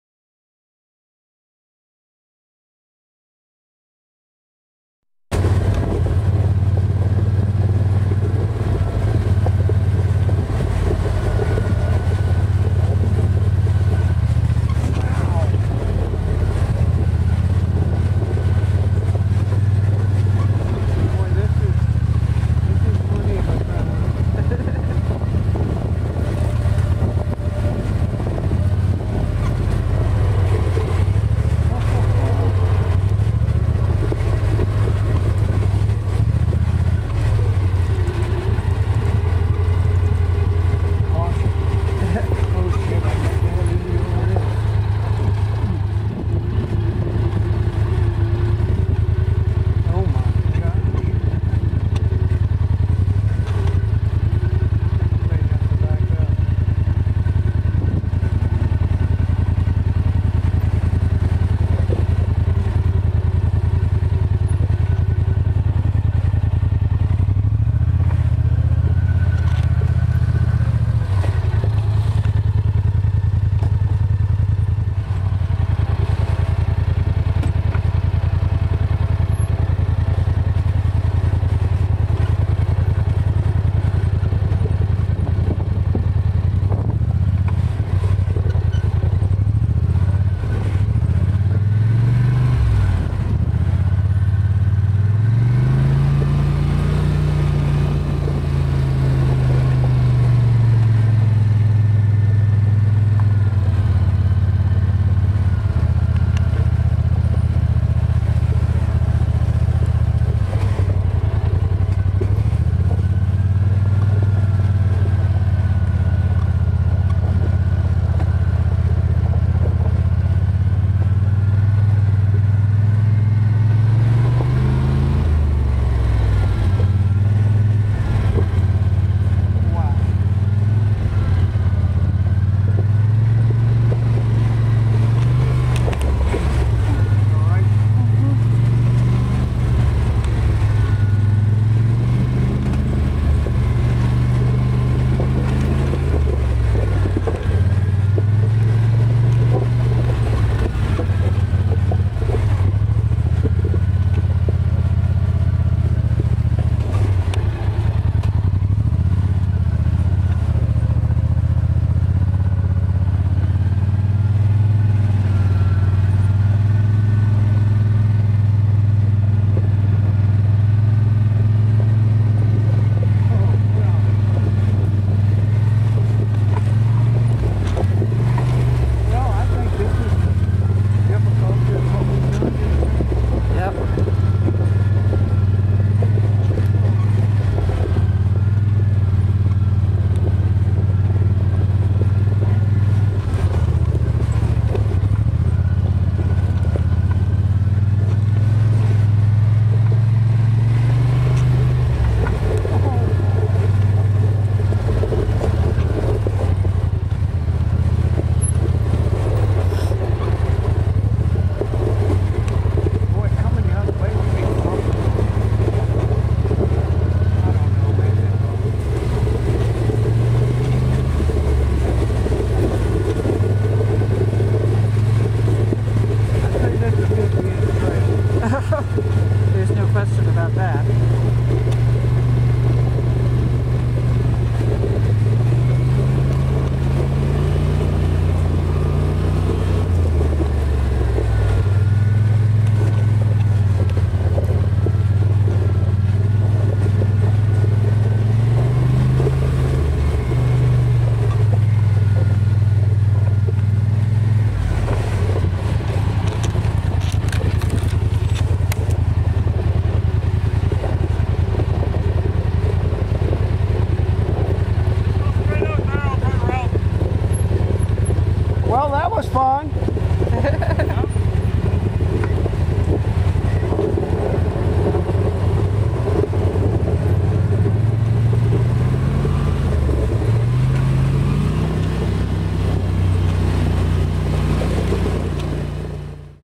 White Sage Trail 2015
My GoPro died on the trail. Sue, riding on the back took this video on a very narrow trail. She did a great job. still picture of her in mirror to start video